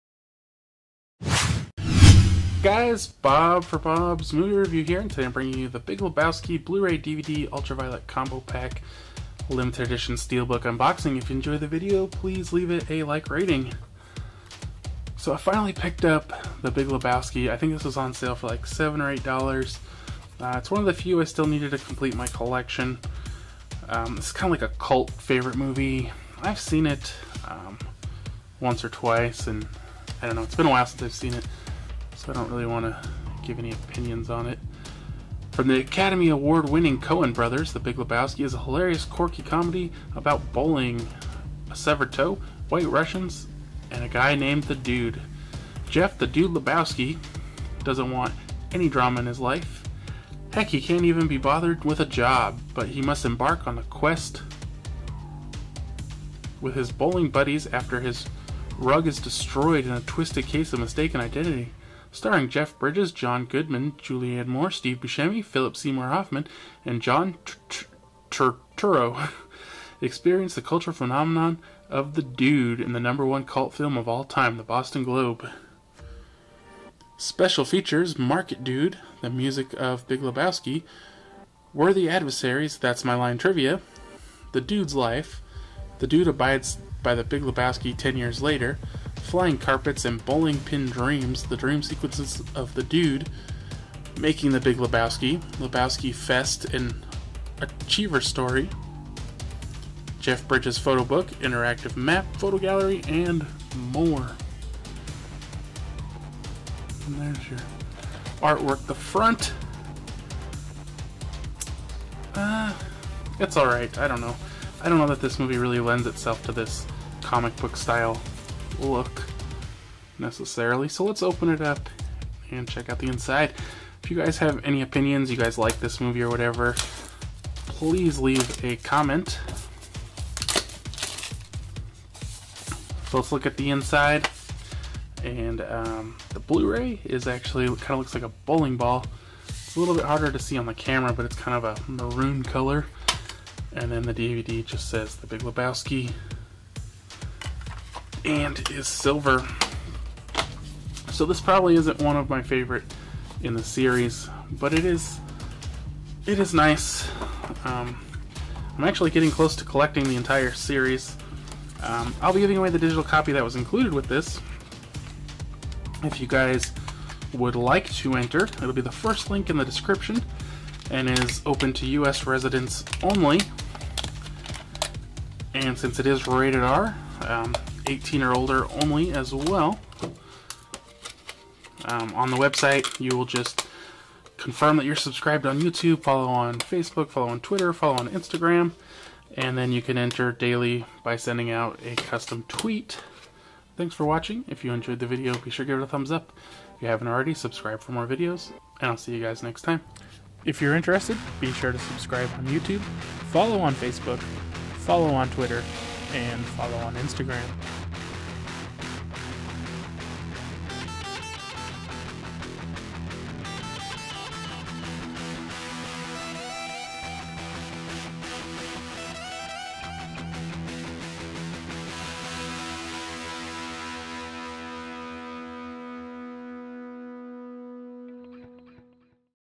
The Big Lebowski Steelbook Unboxing (Giveaway Ended)
The Big Lebowski Limited Edition Comic Steelbook Unboxing

- The Big Lebowski
Rated R , Runtime 117 Minutes
Rating 8.2 /10 , Bob's Rating N / A
Directors Joel Coen , Ethan Coen
Stars Jeff Bridges , John Goodman , Julianne Moore ,
Steve Buscemi , David Huddleston , Philip Seymour
Hoffman , Tara Reid , Philip Moon , Mark Pellegrino ,
Peter Stormare , Flea , Torsten Voges , Jimmie
Dale Gilmore , Jack Kehler , John Turturro